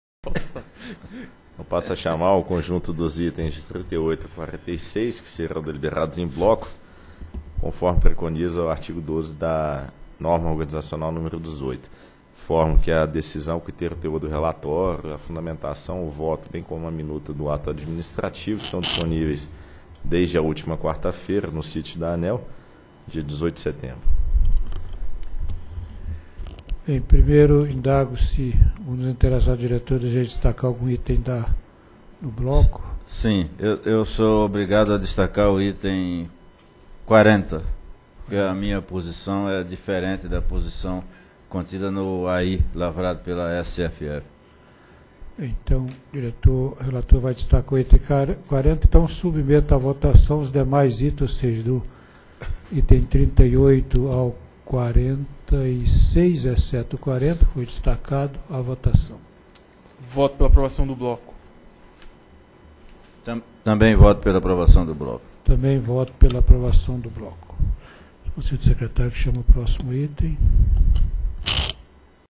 Item 38, 39, 41 ao 46 - 36ª Reunião Pública Ordinária ANEEL 24/09/2013
Os itens 38 a 46 serão deliberados em bloco, conforme o artigo 12 da Norma Organizacional nº 18, revisada pela Resolução Normativa nº 468/2011.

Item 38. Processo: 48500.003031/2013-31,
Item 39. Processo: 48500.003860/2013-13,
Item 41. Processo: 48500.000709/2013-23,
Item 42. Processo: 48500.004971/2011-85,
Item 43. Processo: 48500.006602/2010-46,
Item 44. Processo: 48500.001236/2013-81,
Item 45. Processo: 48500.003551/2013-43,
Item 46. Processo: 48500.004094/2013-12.